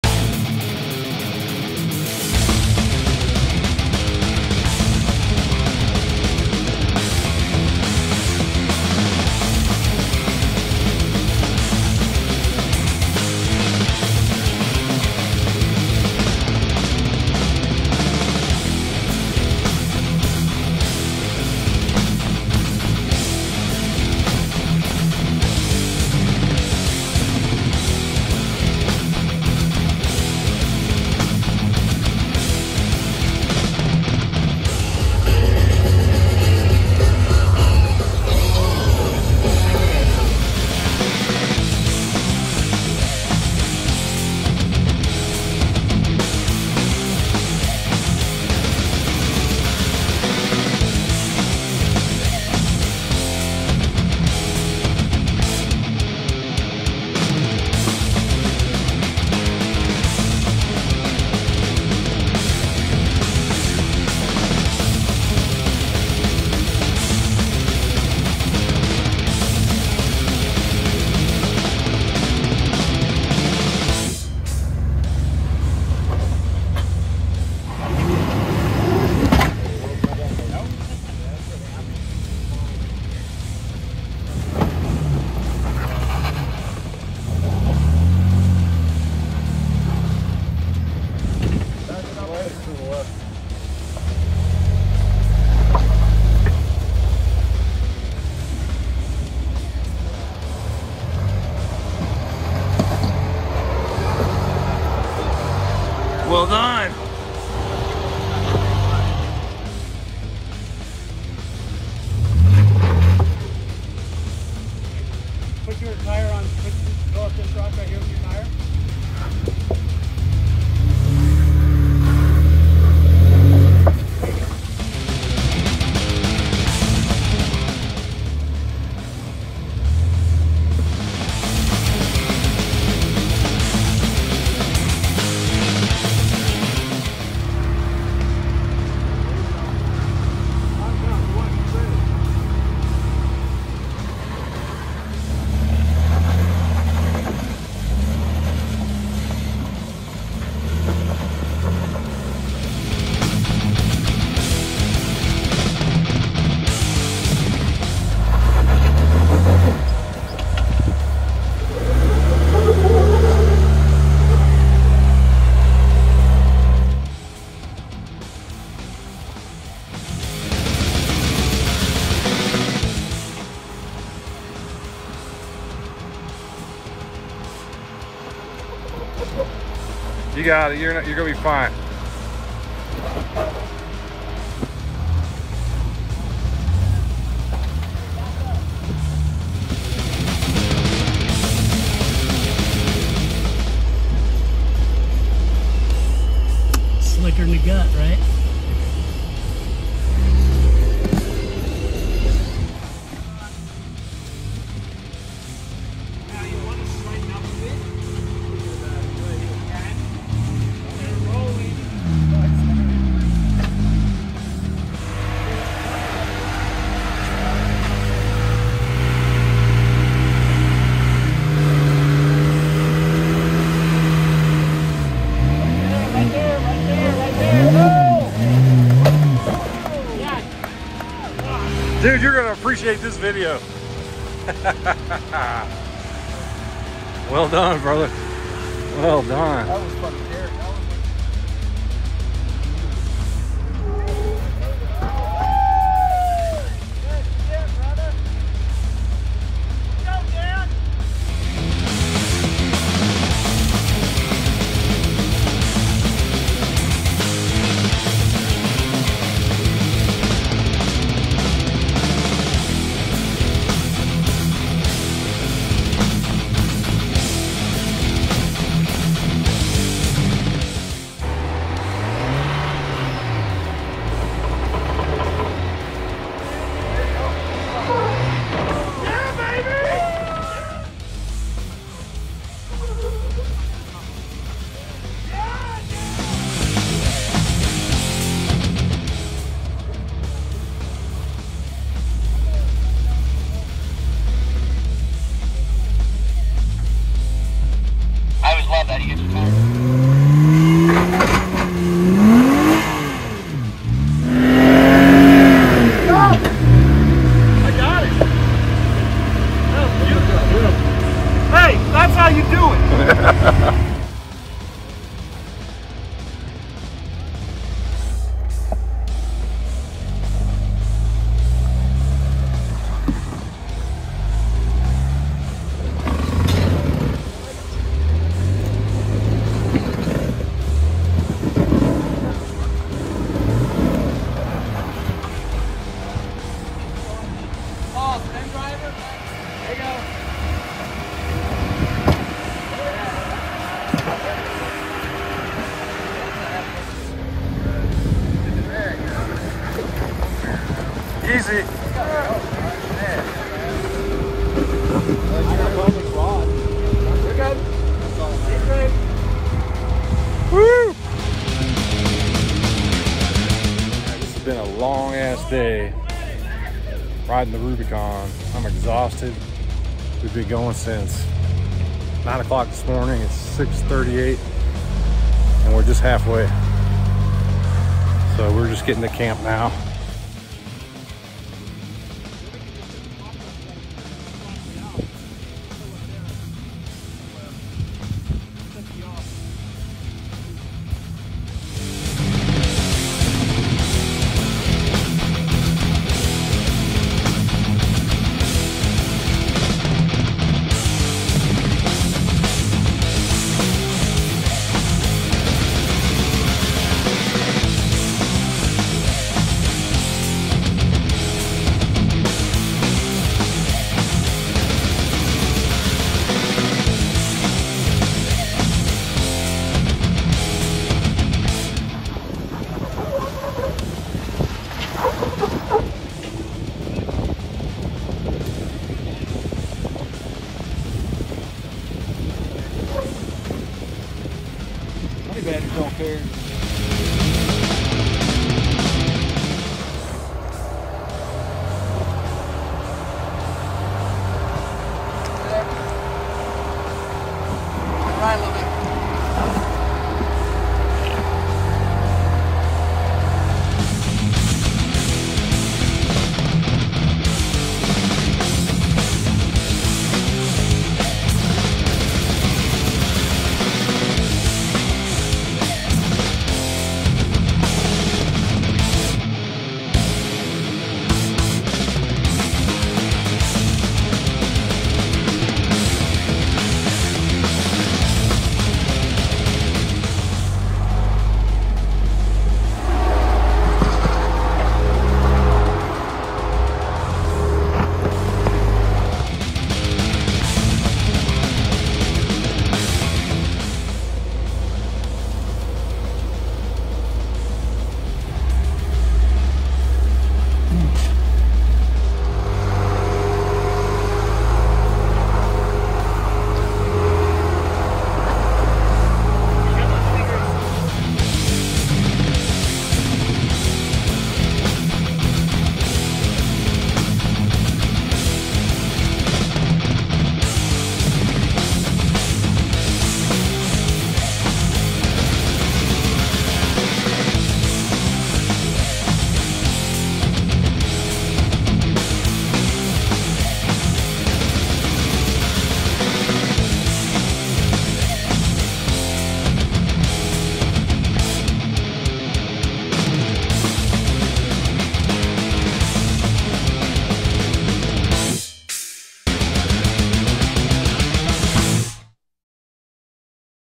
Rubicon Trail with Jeep JL No-BS FB Group
This video highlights our 3-day trip across the Rubicon Trail in California!